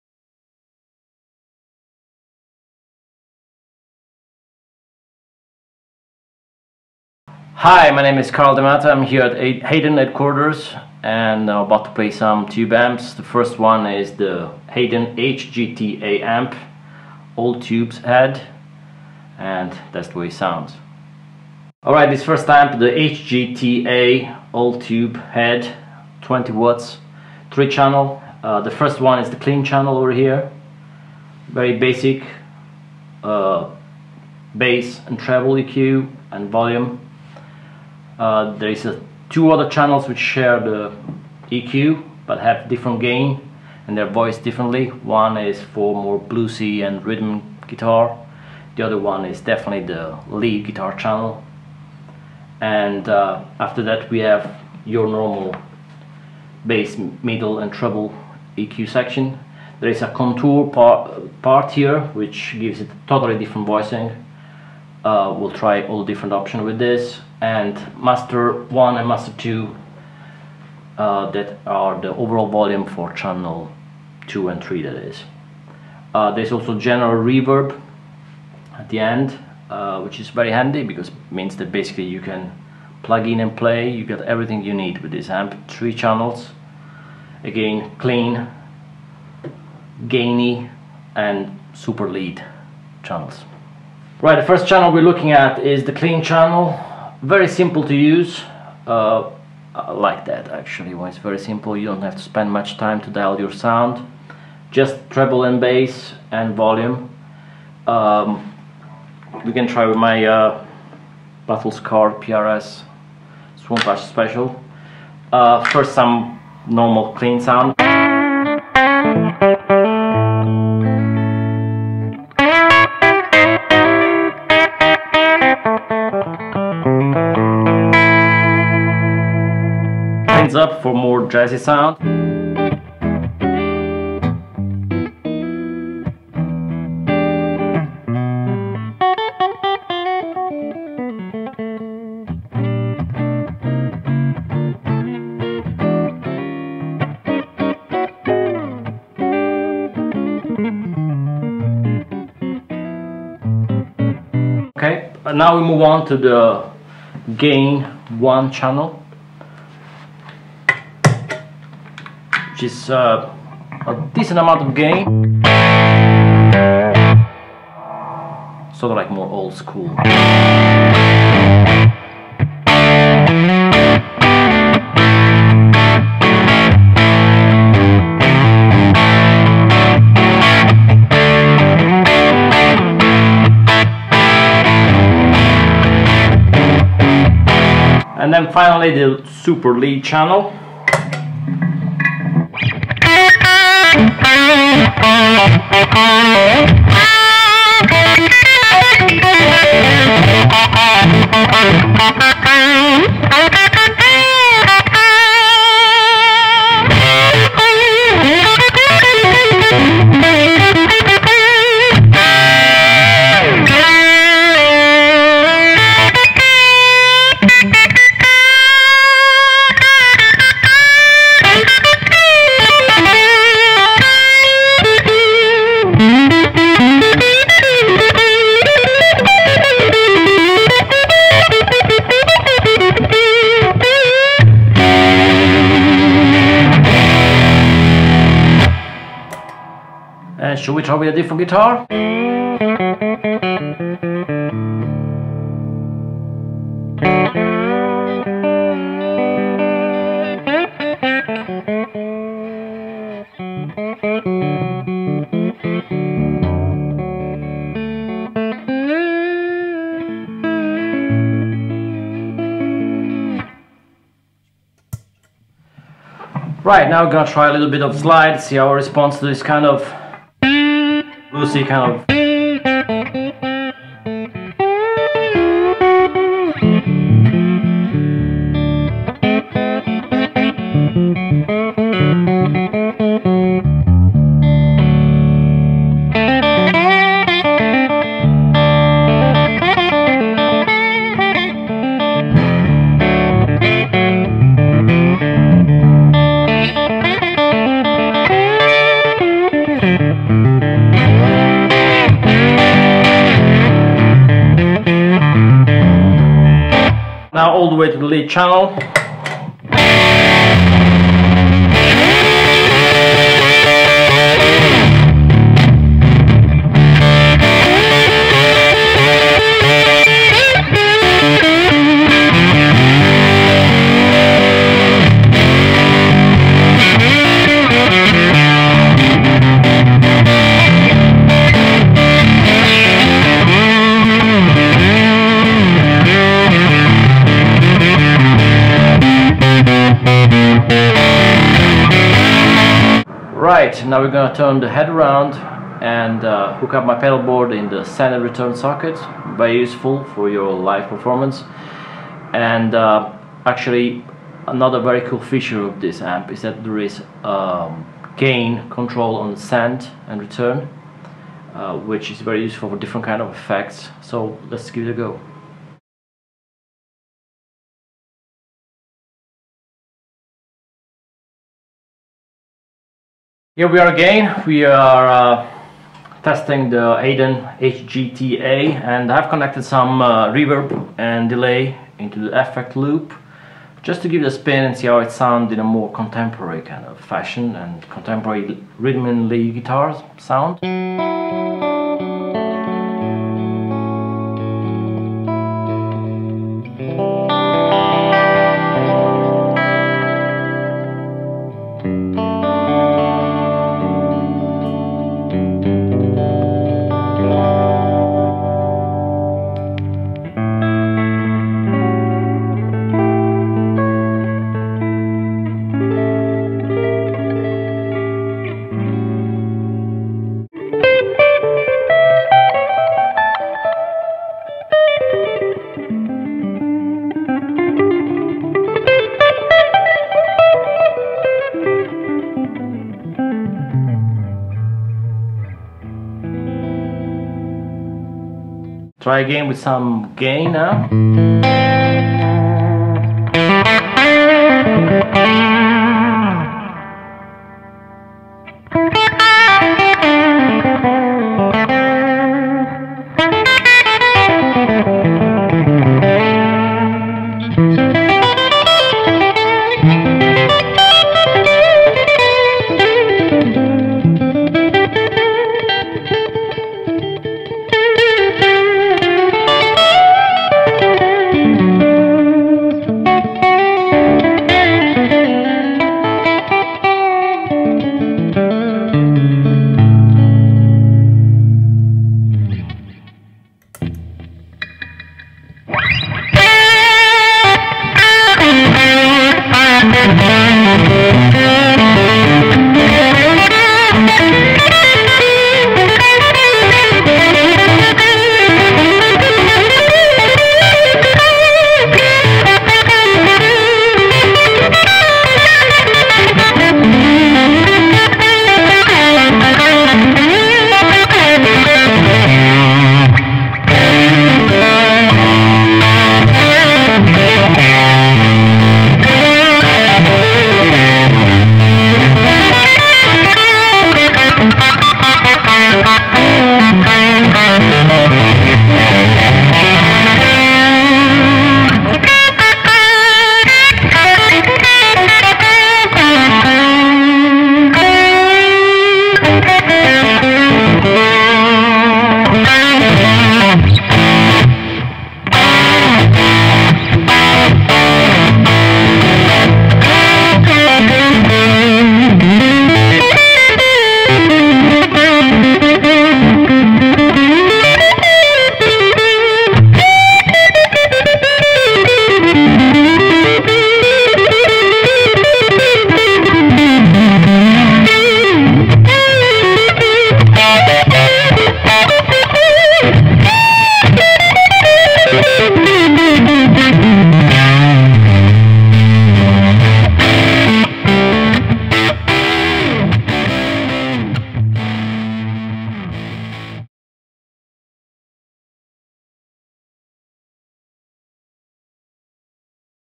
Hayden HGTA 20 Guitar Amp Head Run Through and Demo
A quick run through of the Hayden HGTA 20 Head covering the features and some of the tones available.